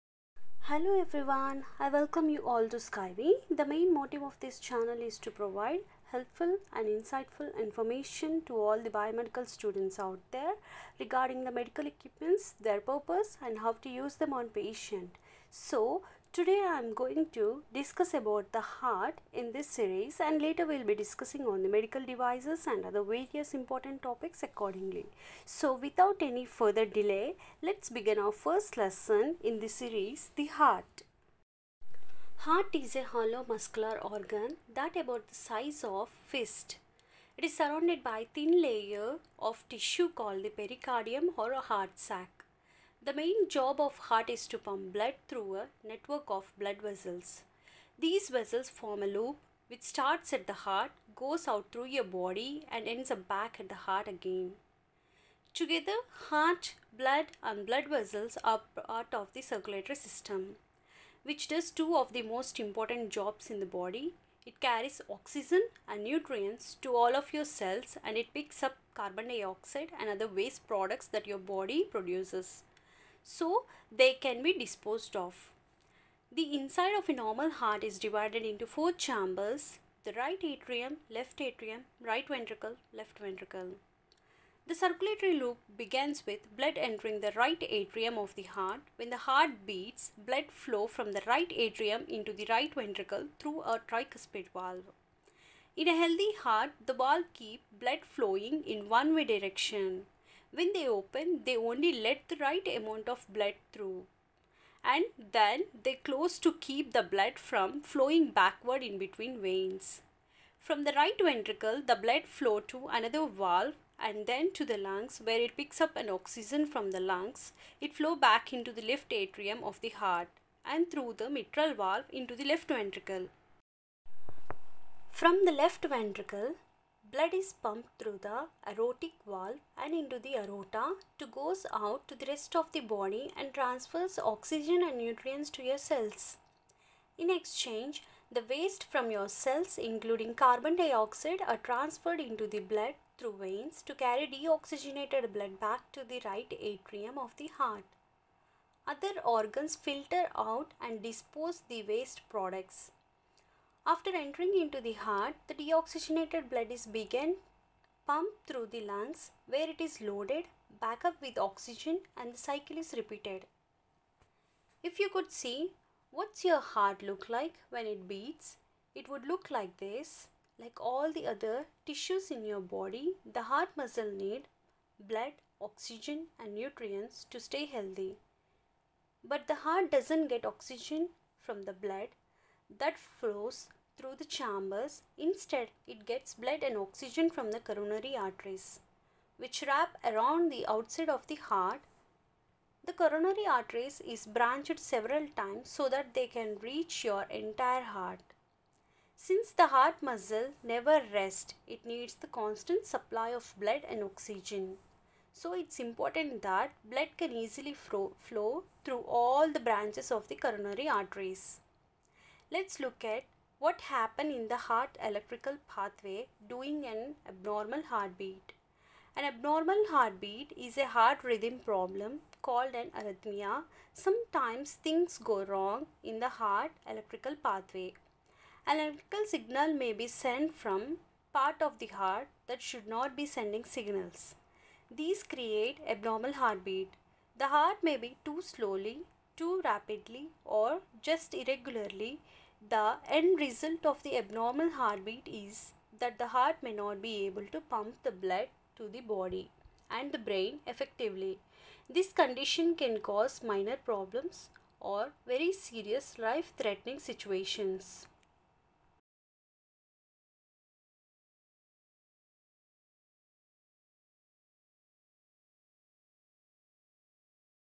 Anatomy of the Heart | Biomedical Engineering | SCI-WI
Heart Anatomy
Your heart is located between your lungs in the middle of your chest, behind and slightly to the left of your breastbone (sternum). A double-layered membrane called the pericardium surrounds your heart like a sac. The outer layer of the pericardium surrounds the roots of your heart's major blood vessels and is attached by ligaments to your spinal column, diaphragm, and other parts of your body.

Stay Tune..
SCI - WI - Biomedical Engineering